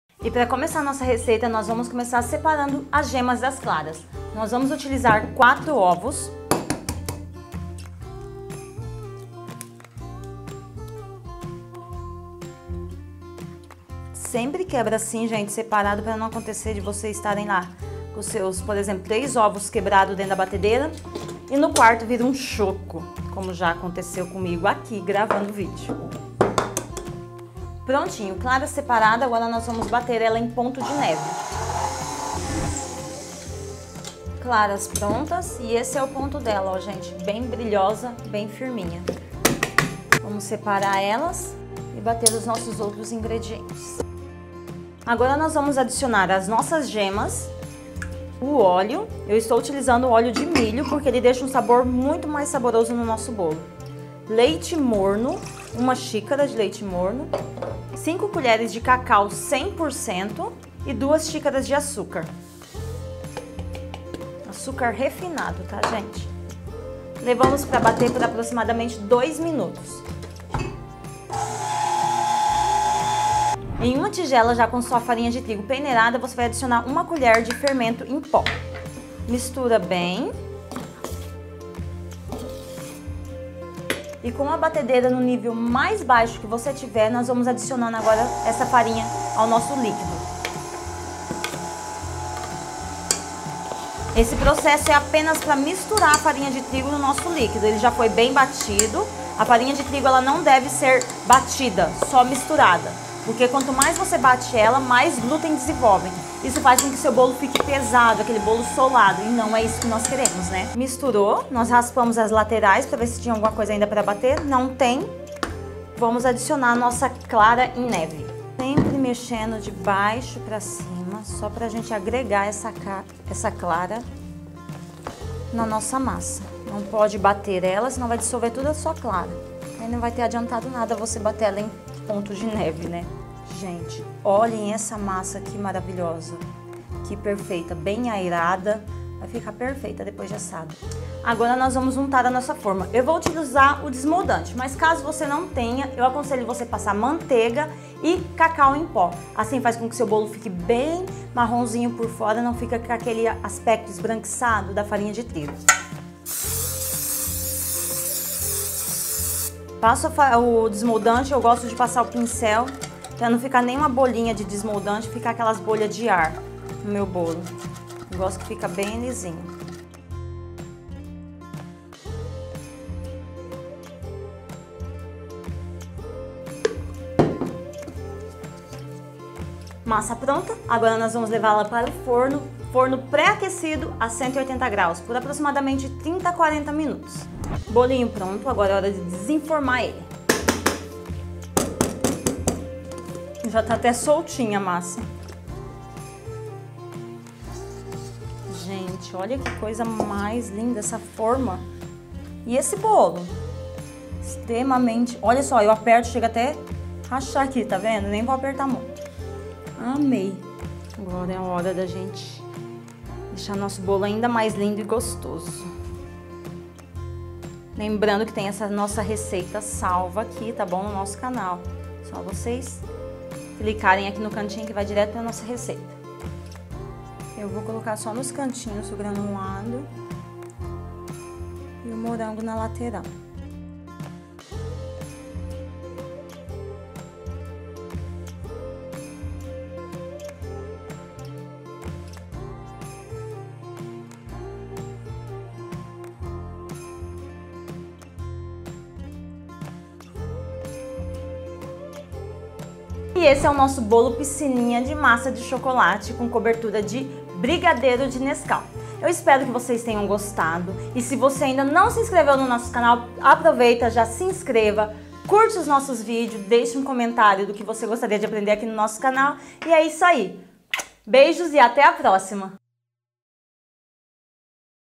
Massa para Bolo de Chocolate Profissional | Piscininha
Oi genteee!!! 👩‍🍳 Aprenda comigo essa Receita de Bolo de Chocolate Fofinho (Bolo Piscininha) que é sucesso em vendas aqui em meu ateliê. 💲😍

INGREDIENTES do Bolo de Chocolate:
4 ovos
1 e 1/2 xícara de açúcar refinado
1/2 de óleo
3 colheres de cacau 50%
1 xícara de leite
3 xícaras de trigo
1 colher de fermento em pó

0:00 - Oi genteee!!! E no vídeo de hoje, Receita de Bolo de Chocolate Fofinho
0:05 - Ingredientes e Modo de preparo
2:21 - Como untar forma para bolo
3:07 - Bolo Piscininha Pronto
3:30 - Cobertura de Brigadeiro Cremoso
4:14 - Encerramento